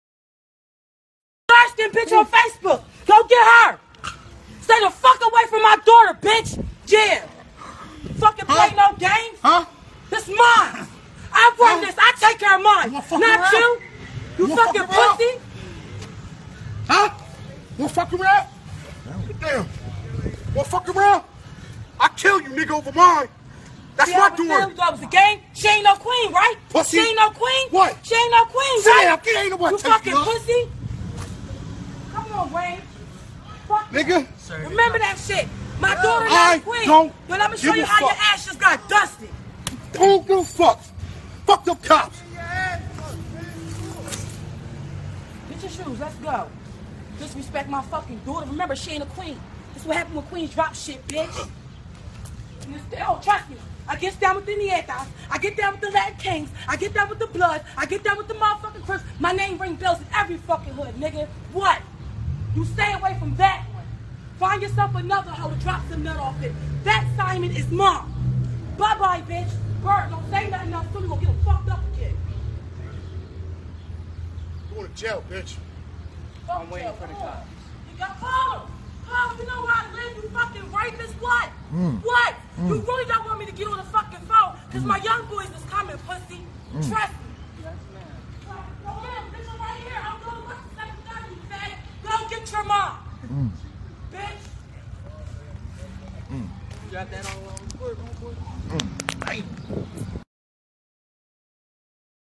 Parents Catch Guy Who ^^Disrespected^^ Their Daughter and Deliver Some Hood Justice
daughter of the parents, man disturbed on facebook her .And Justice for him so good.